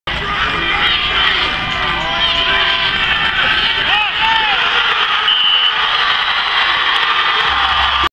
North Harford at Fallston (Football) 10-26-12-9
North Harford's Matt Precht sacks Brandon Weinhold in the fourth quarter.